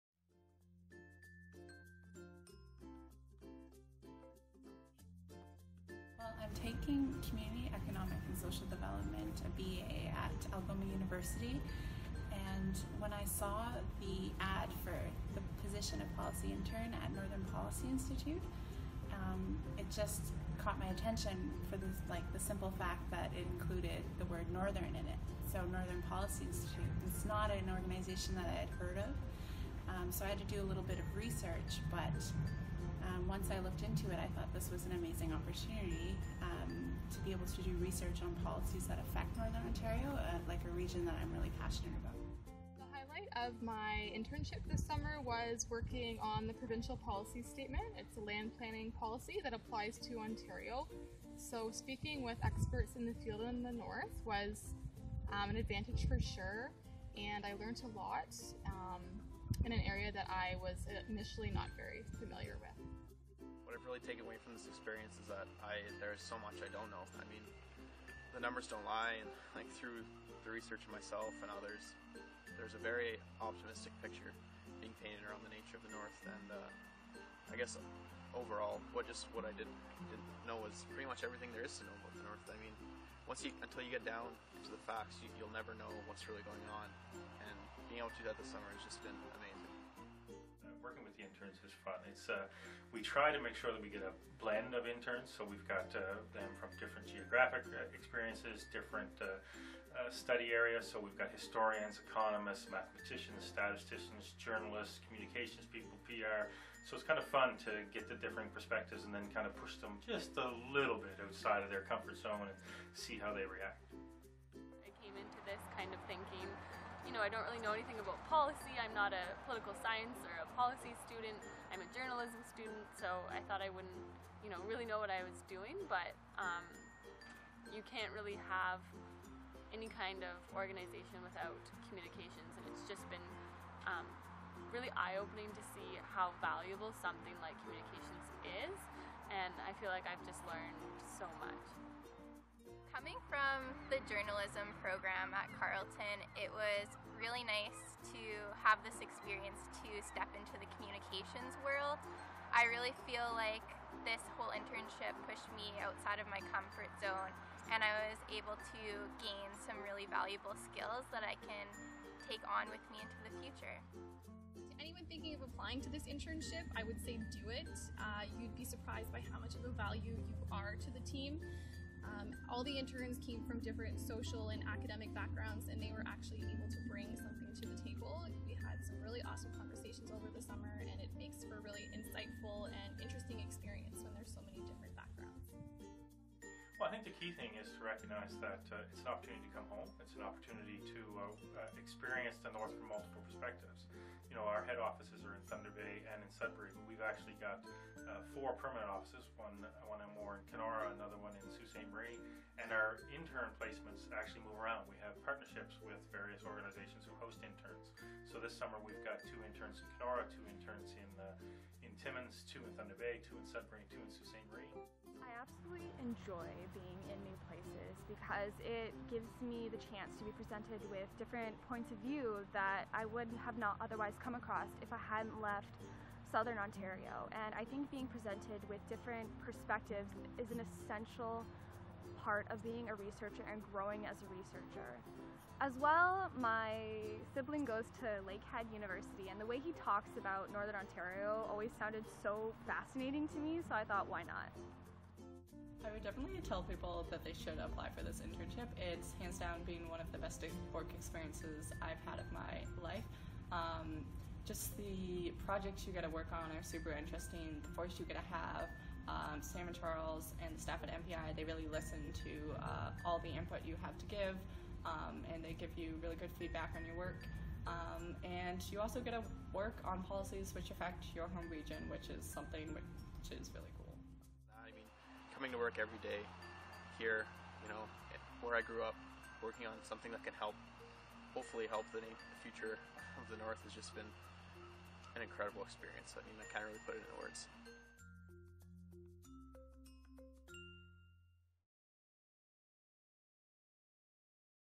Experience North Program 2016
Get to know Northern Policy Institute's summer placement program, Experience North, through the eyes of past students.

Each January, NPI accepts applications for positions in policy*, communications, and public relations. for more information!

*Policy positions coming soon.